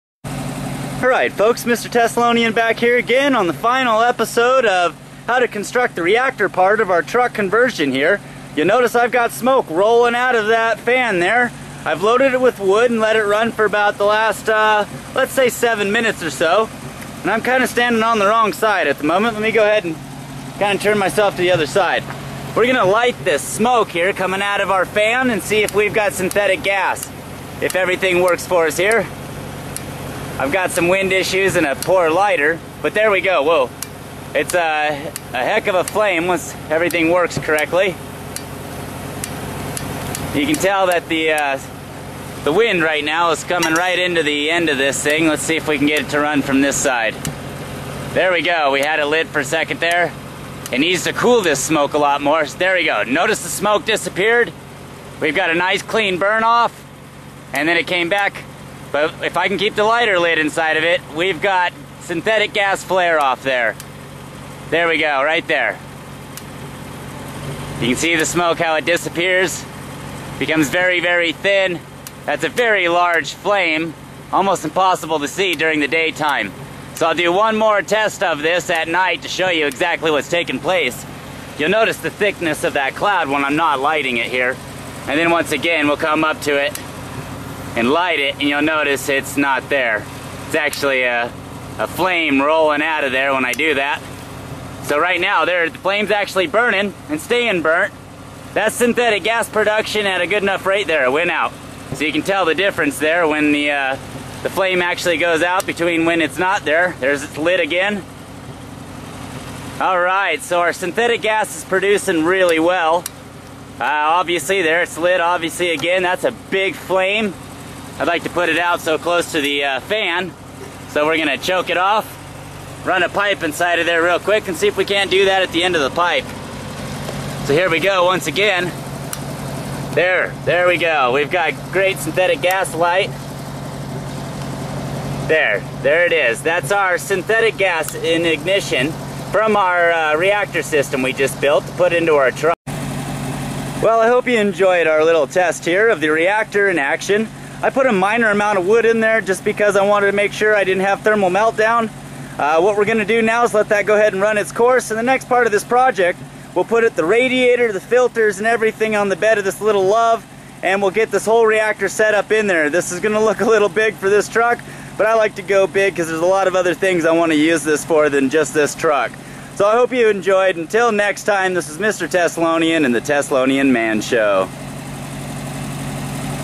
Gasifier Truck How-To part 5
This is step by step how to build a Wood Gasifier for your truck or car.